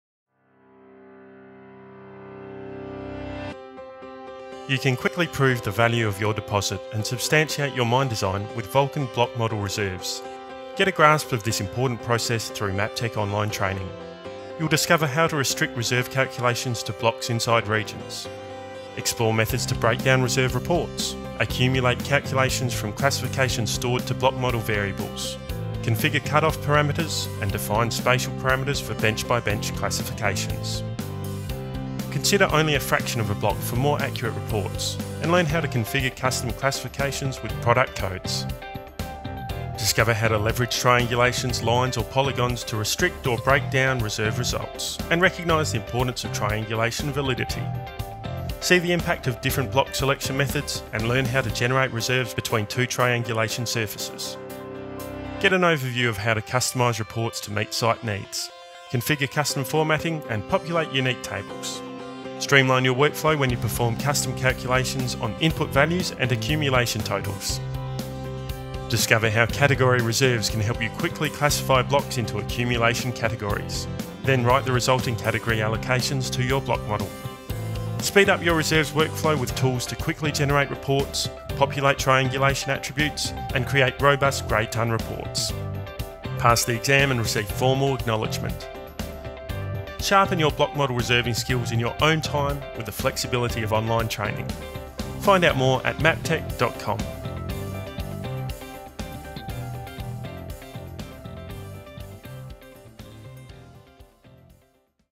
Learn about the Block Model Reserves online training course
Sharpen your block model reserving skills with this Maptek Online Training course. See how to quickly prove the value of your deposit and substantiate your mine design.